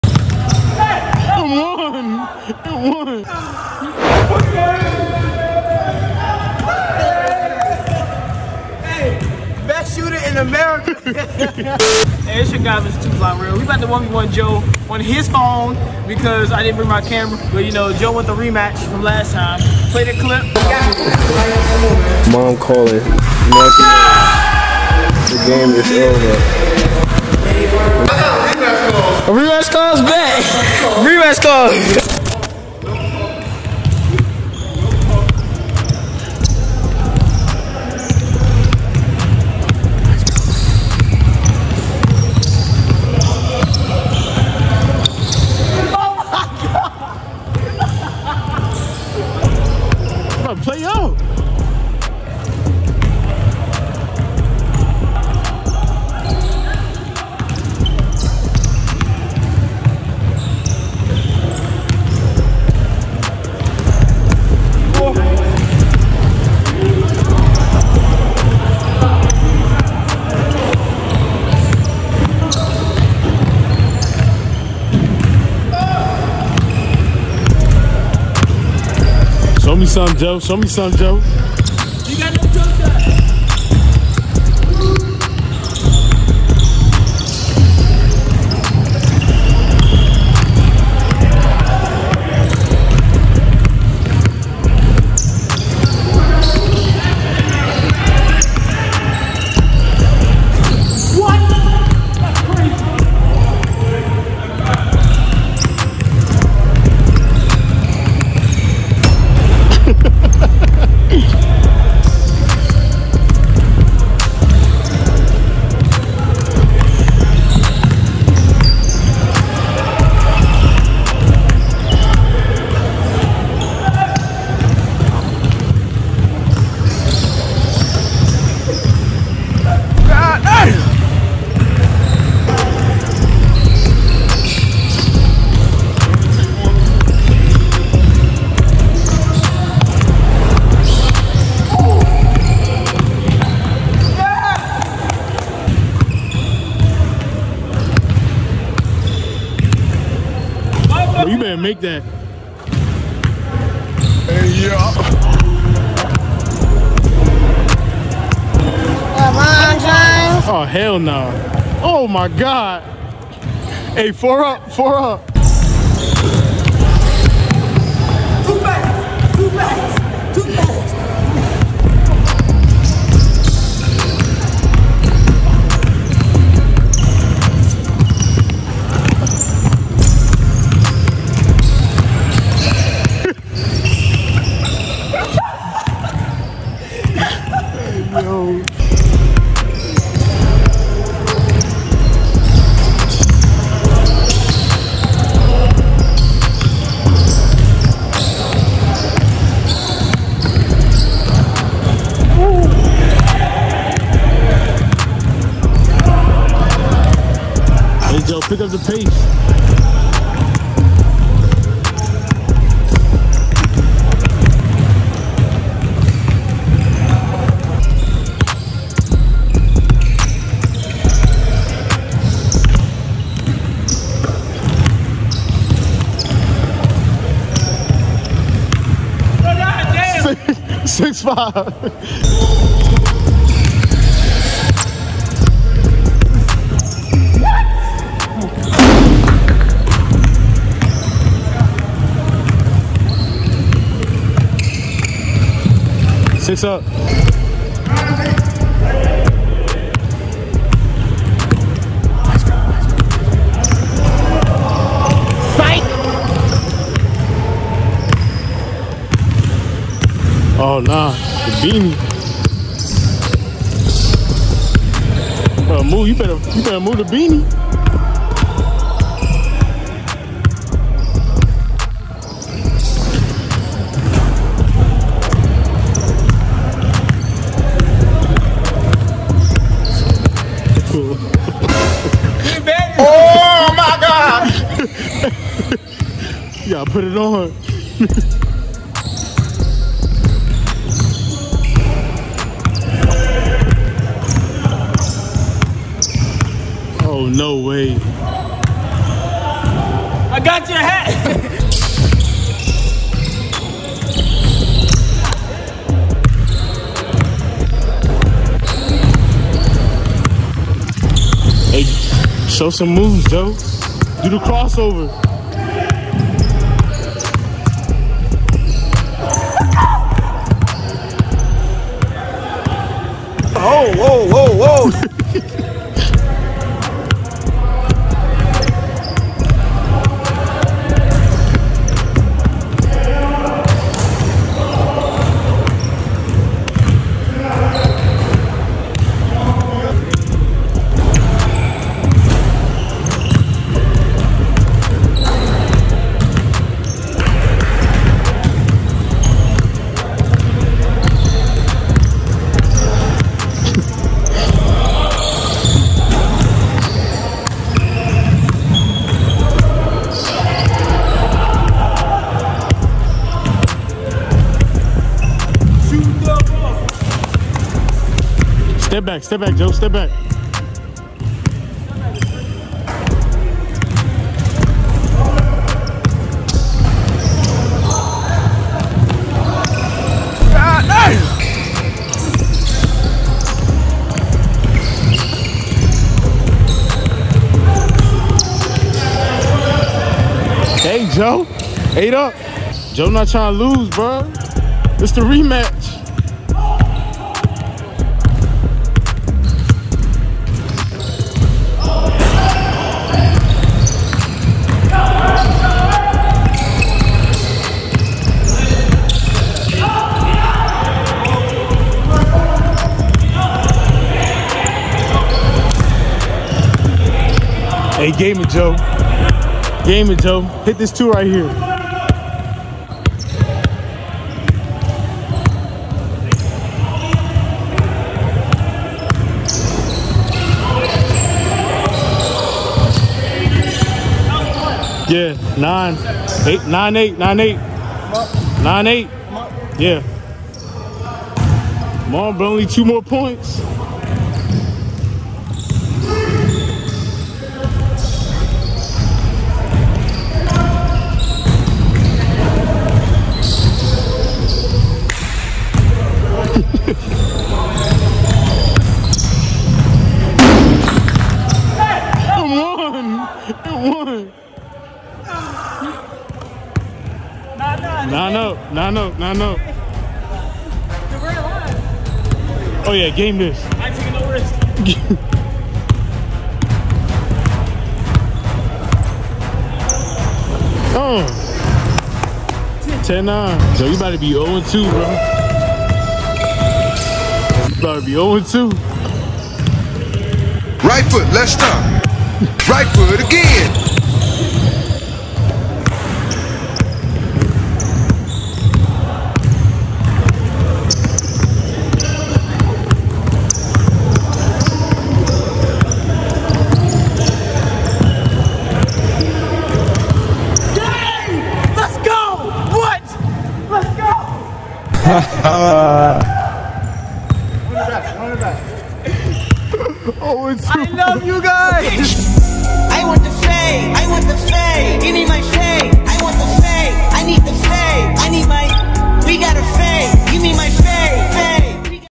1v1 Basketball Rematch Vs 6'3 240 Athlete
I 1v1'd in a rematch vs a 6'3 240 pound big man after beating him by 2 points last year.